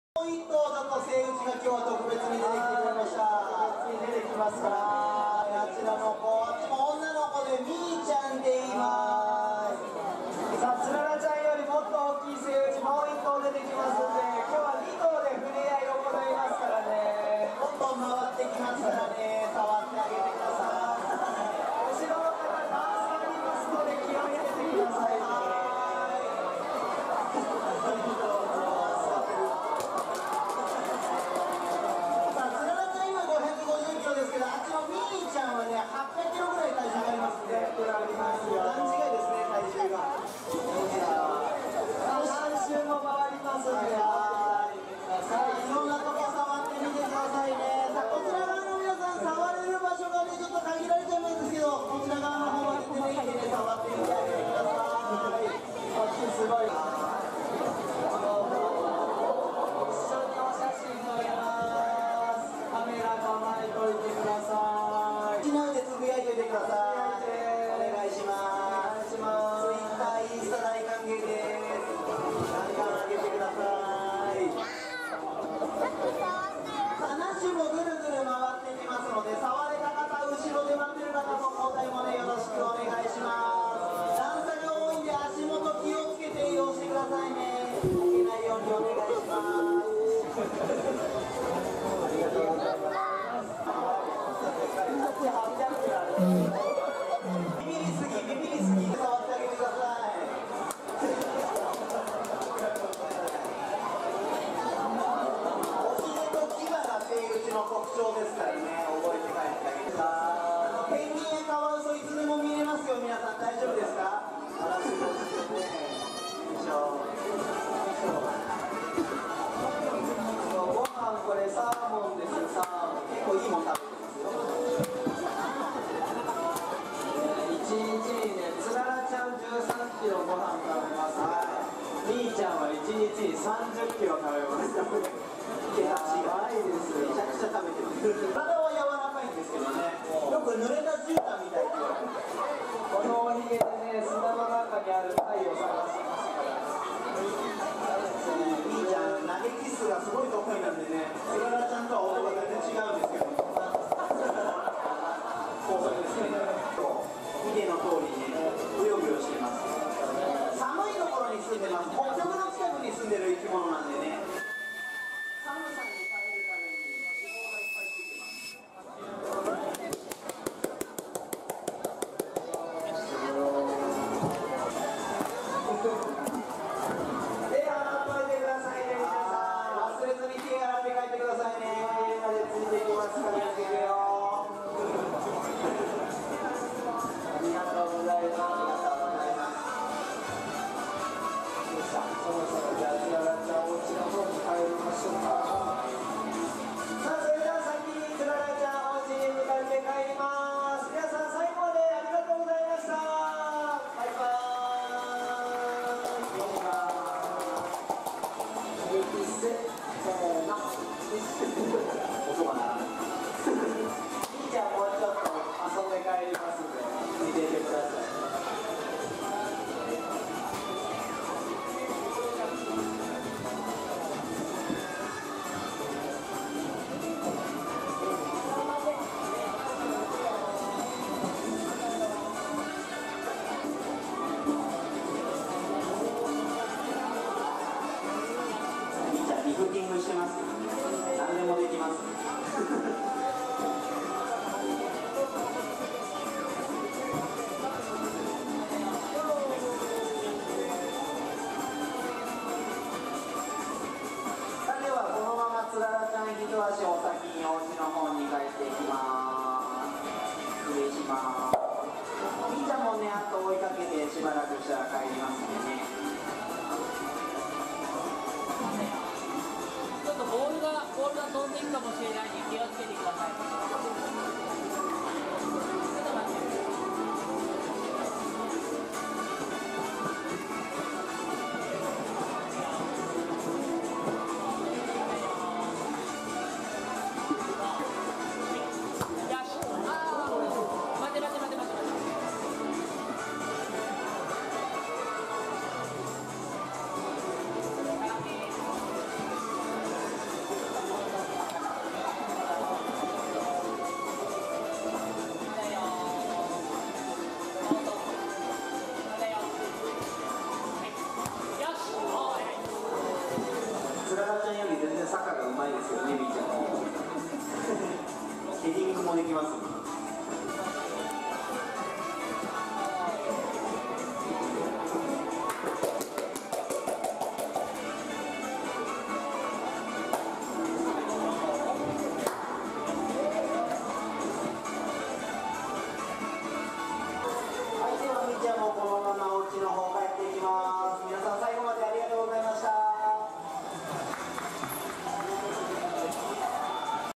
セイウチのミーちゃんとツララちゃんのふれあいタイム@鳥羽水族館
大分のうみたまごから妊活のためやってきているセイウチのミーちゃんがふれあいタイムに！
うみたまごでもショーをやっていたミーちゃんとおっとりマイペースのツララちゃん。レアなコンビです